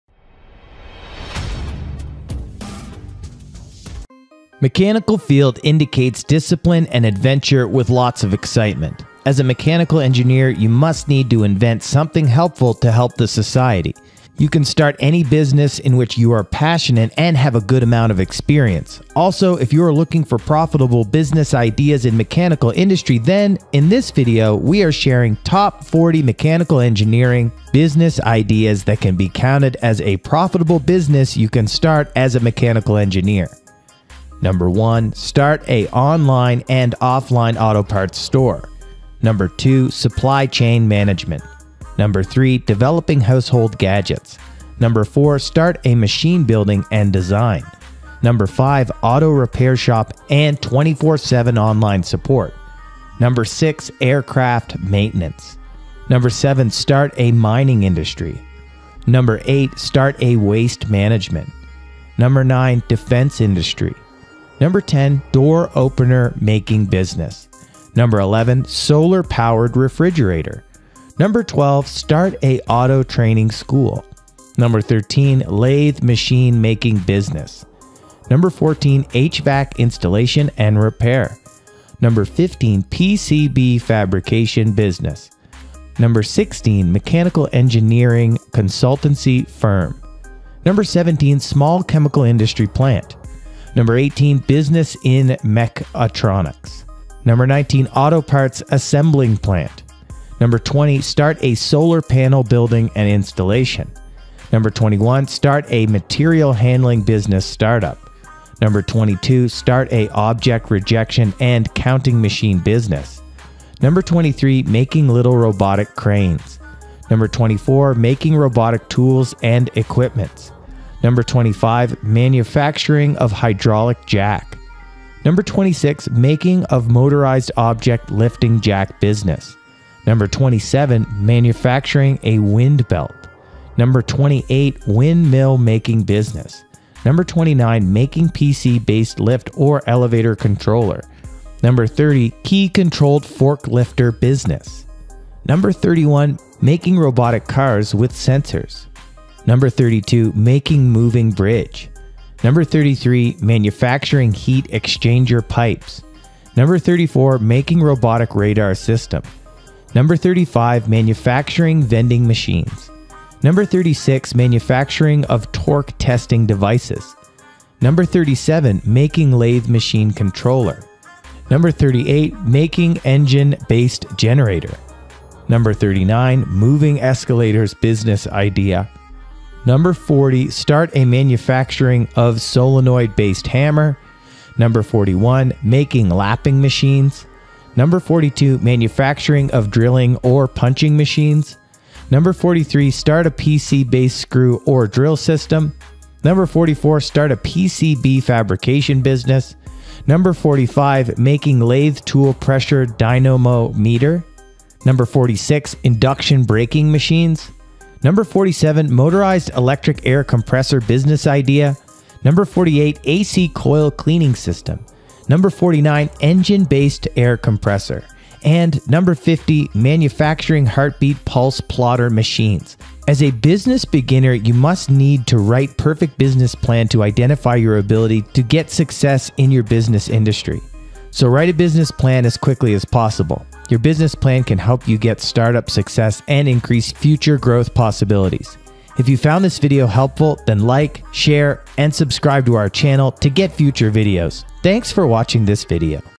Top 40 Profitable Business Ideas in Mechanical Engineering Industry
Get 40 profitable business ideas in mechanical engineering industry.
Mechanical Engineering Business Ideas for starting your own business.